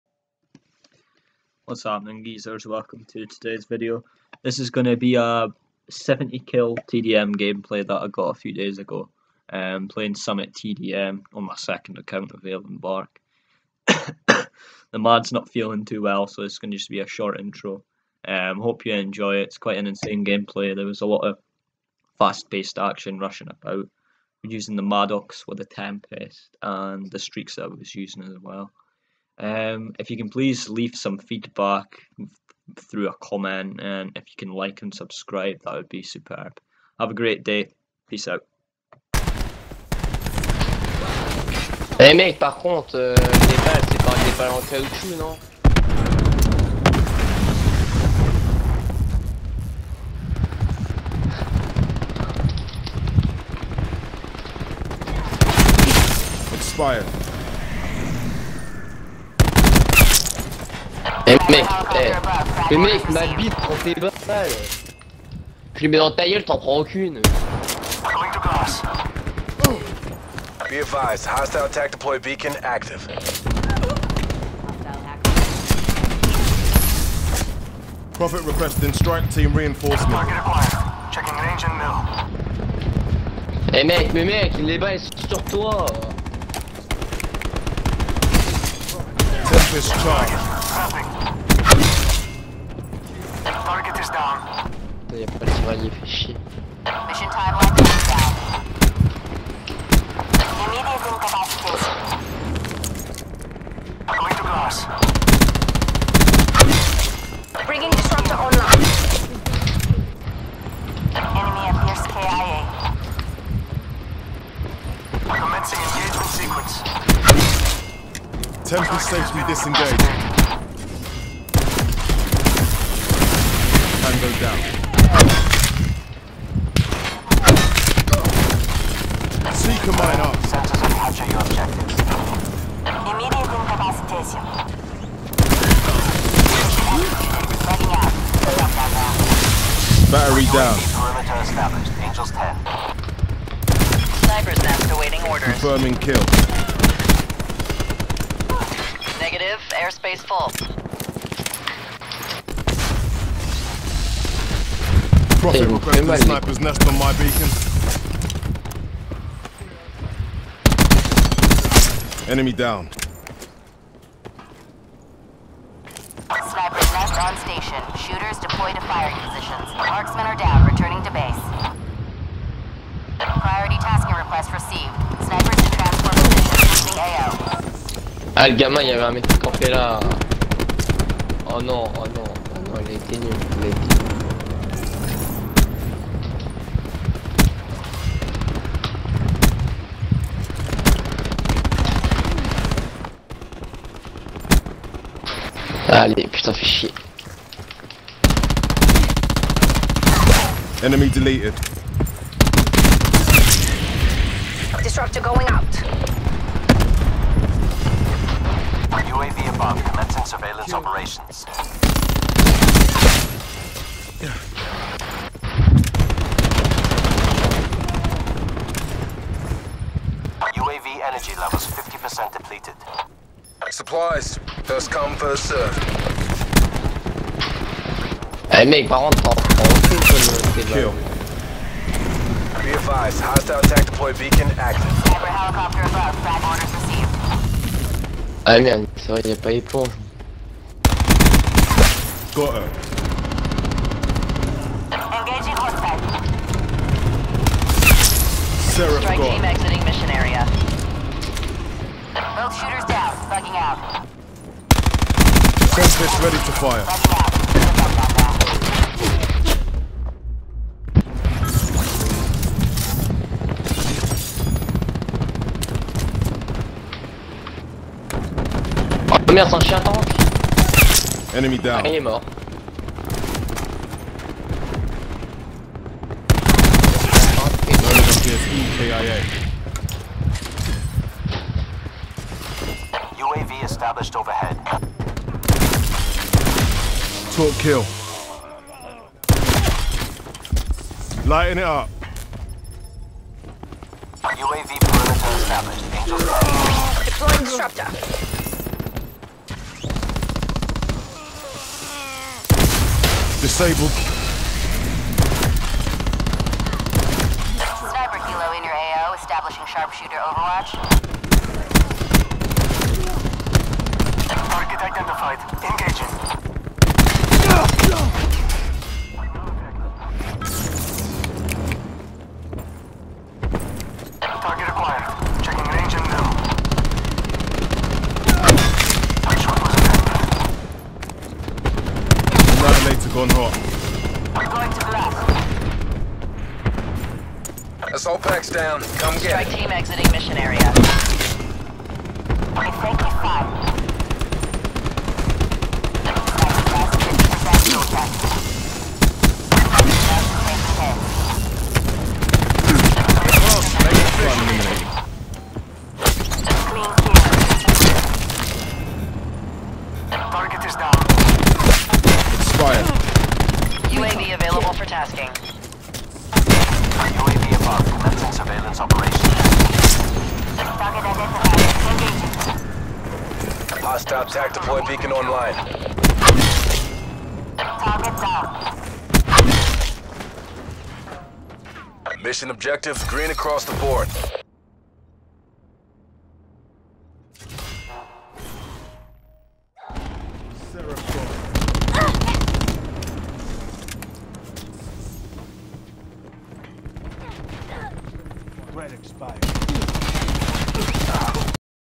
5 Sniper Nests / Quad Strike Team / 70-2 TDM
Welcome to a new video. 70 Kill TDM I got on my second account.
I actually dropped 5 Snipers Nests & 4 Strike Teams in one Team death match lol.
Have a super day, regards - Glxve.